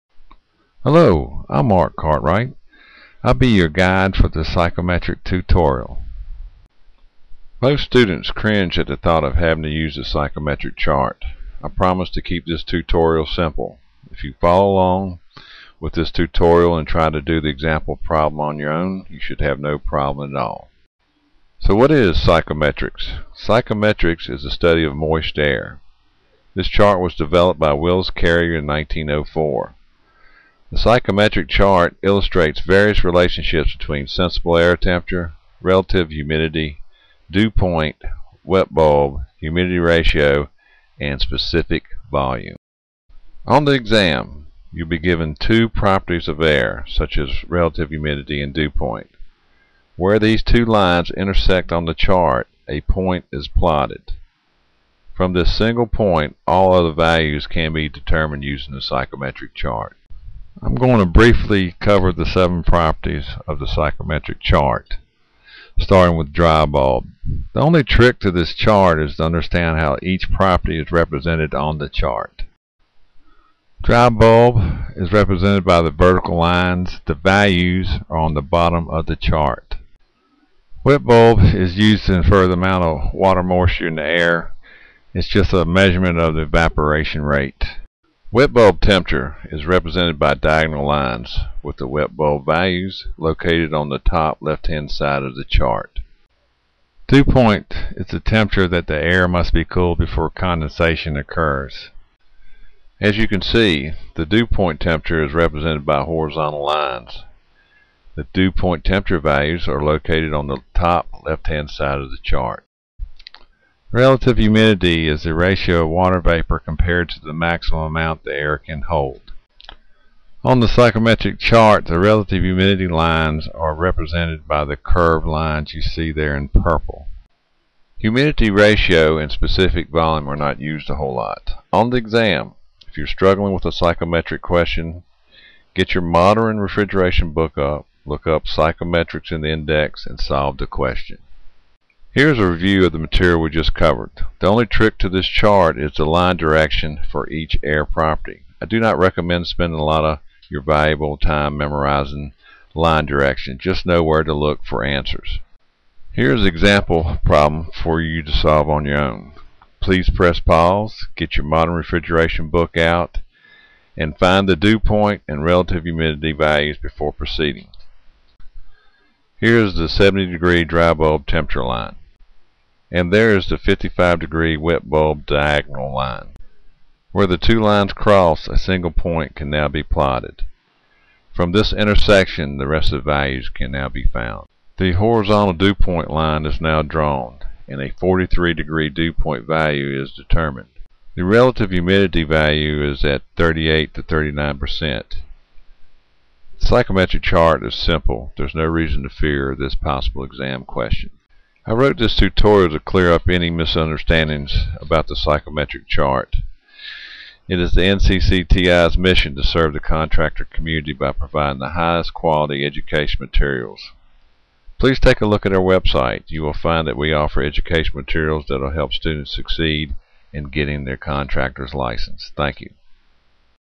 Psychrometric Chart Simplified.flv
Short Tutorial for North Carolina and Virginia Heating Contractor Examinees. This Tutorial simplifies the Psychometric Chart in less than 4 minutes.